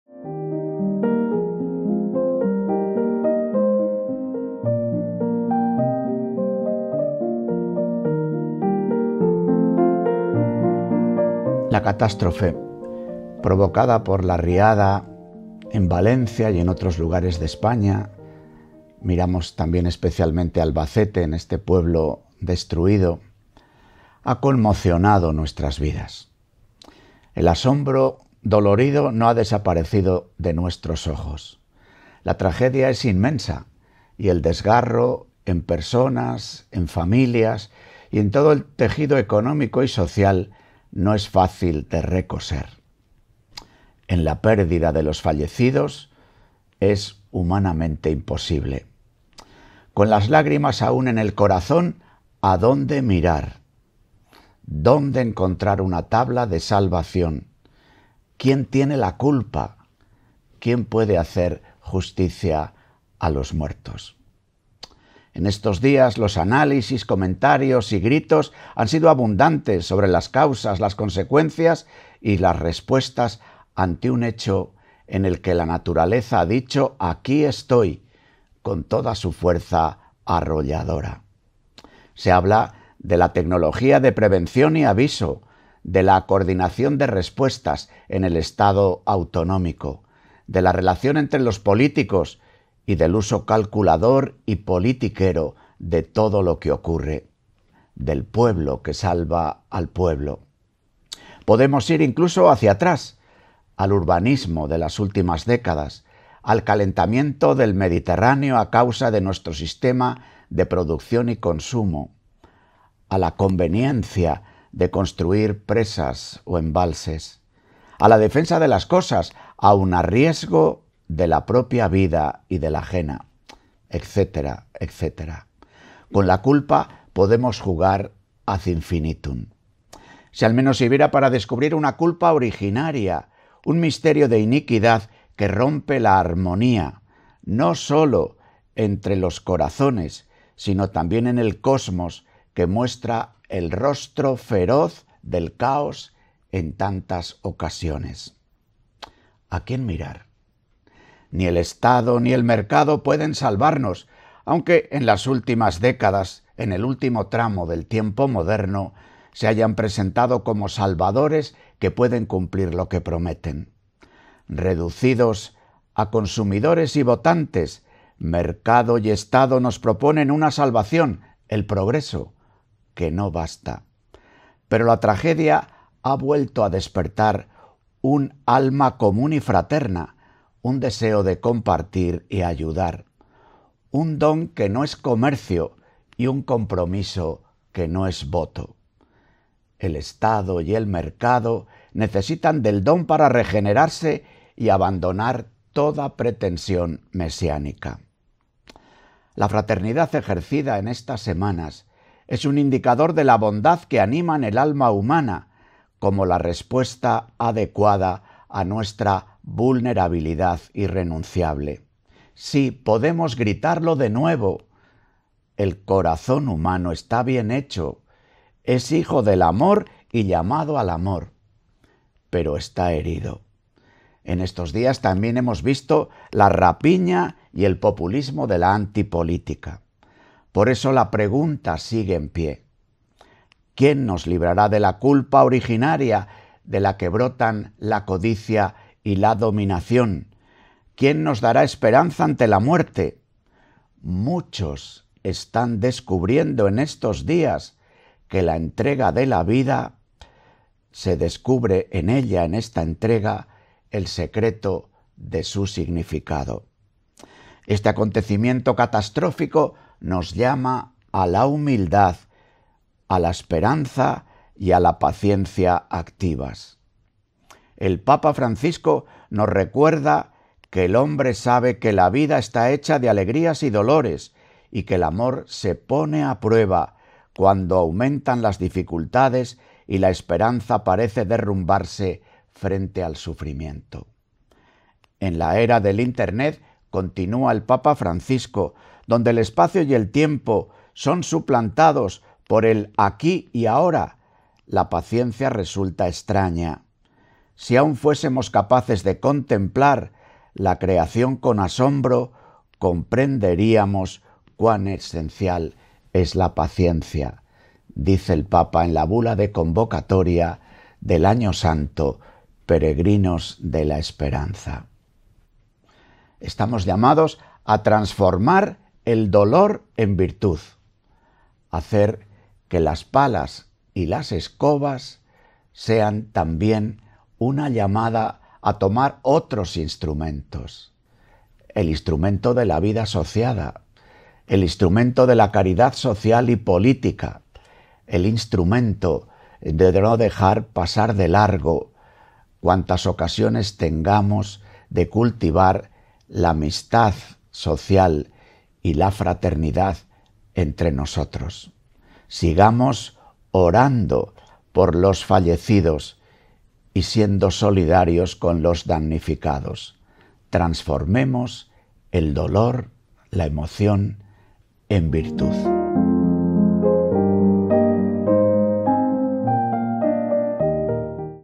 Transformar el dolor en virtud _ Carta Pastoral de Mons. Luis Argüello
Carta pastoral de Mons. Luis Argüello, arzobispo de Valladolid para la segunda quincena del mes de noviembre de 2024